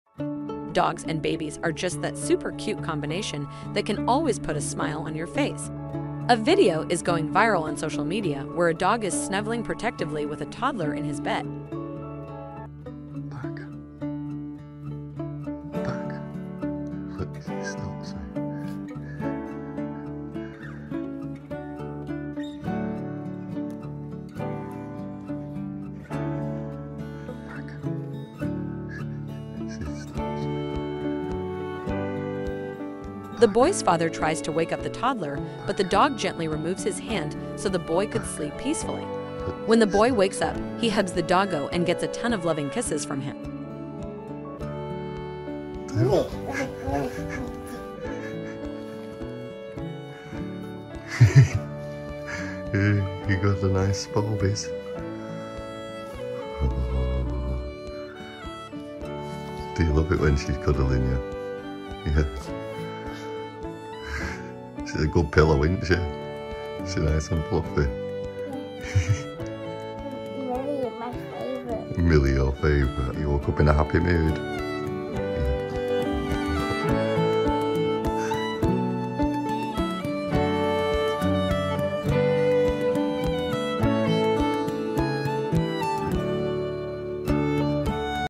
Protective Husky Snuggles With Toddler
They are literally the cutest 😍